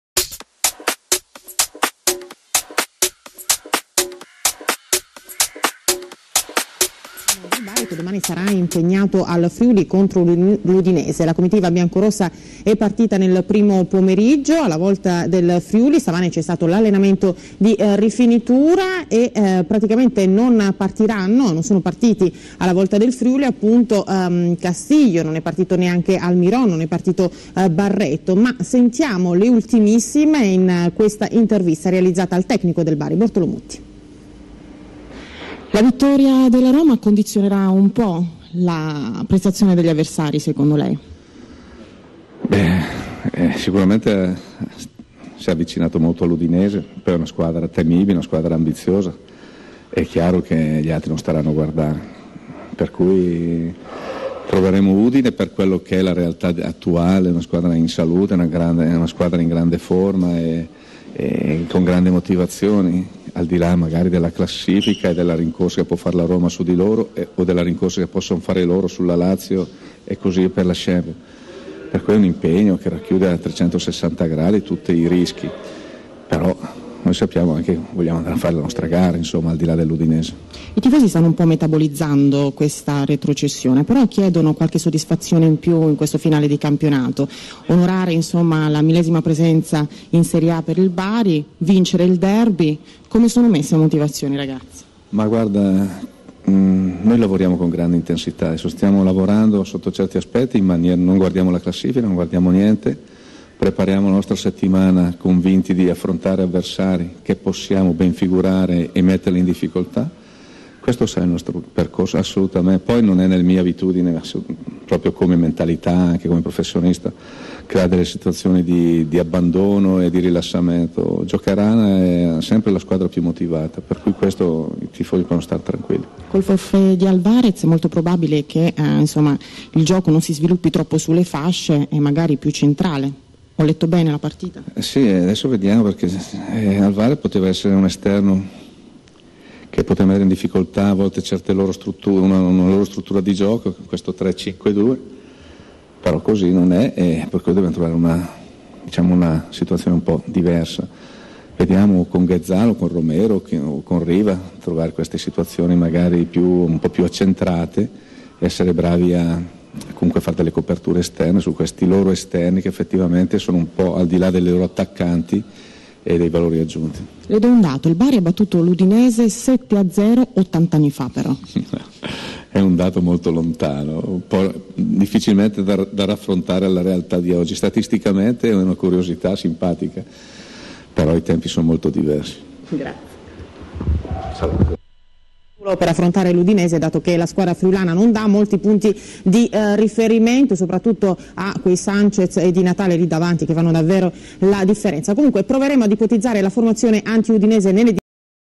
Mutti - Lavoriamo per dare soddisfazioni
Le parole alla vigilia di Udinese Bari